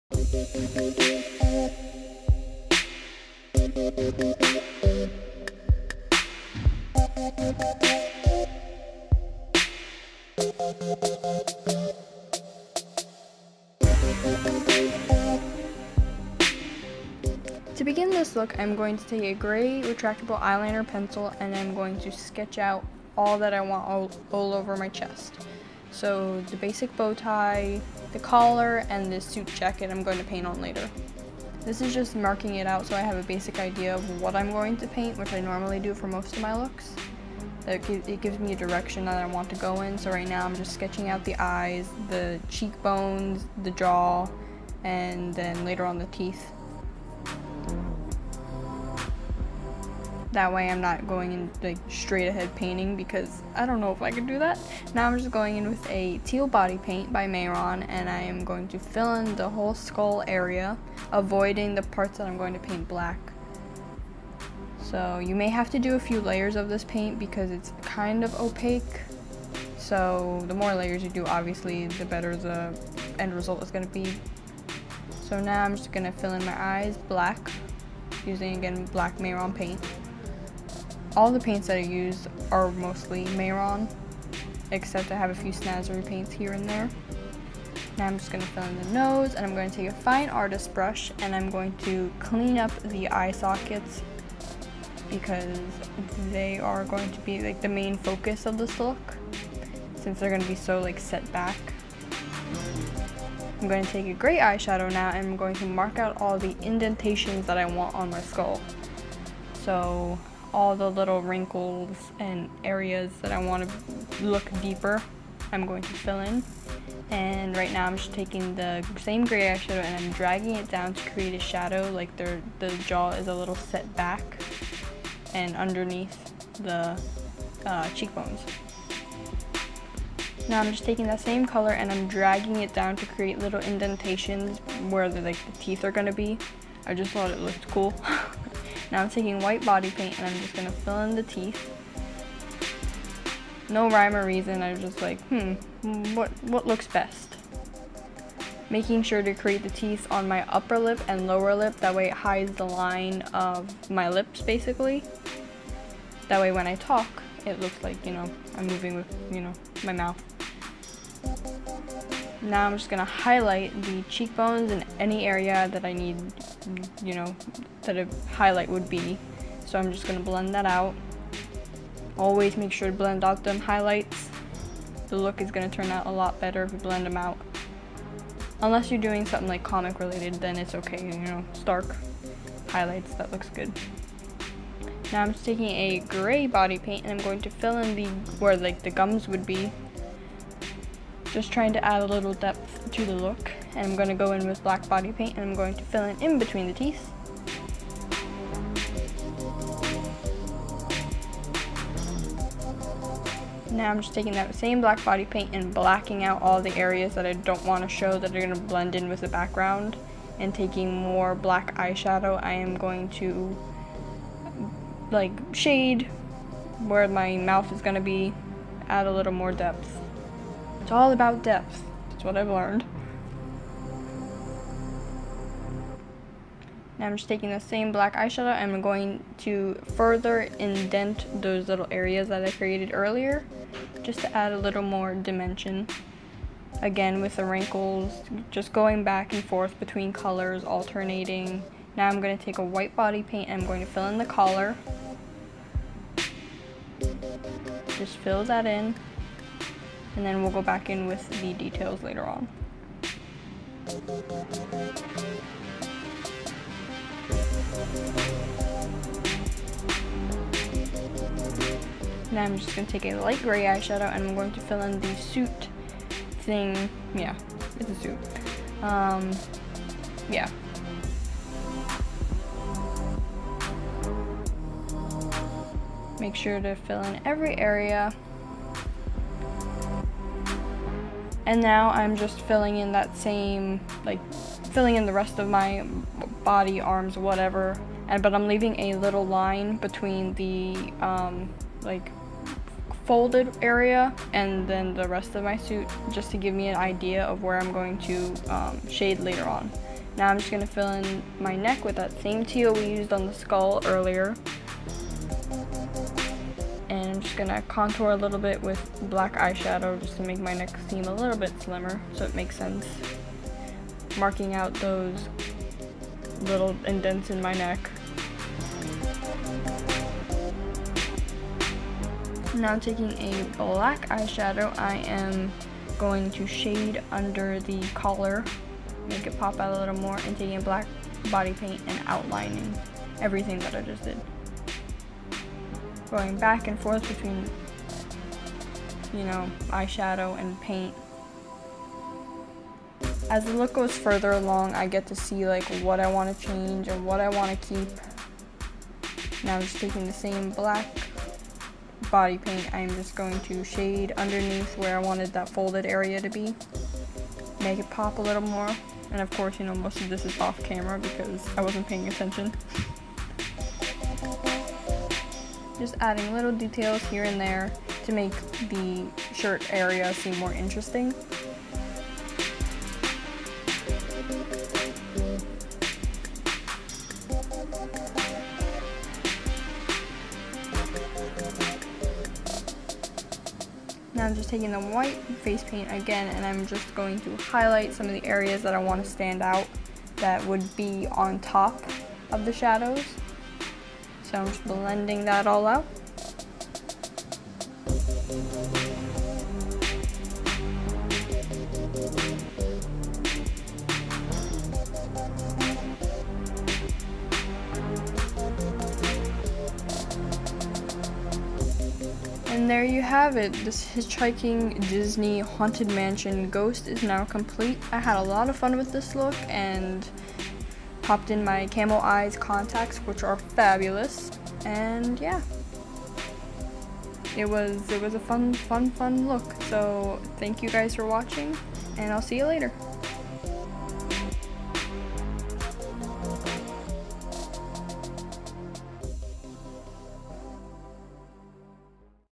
Hitchhiking Ghost
Everyone loves a good Hitchhiking Ghost! I haven't seen many body painted hitchhiking ghosts from Disney's Haunted Mansion so I thought I would create my own version!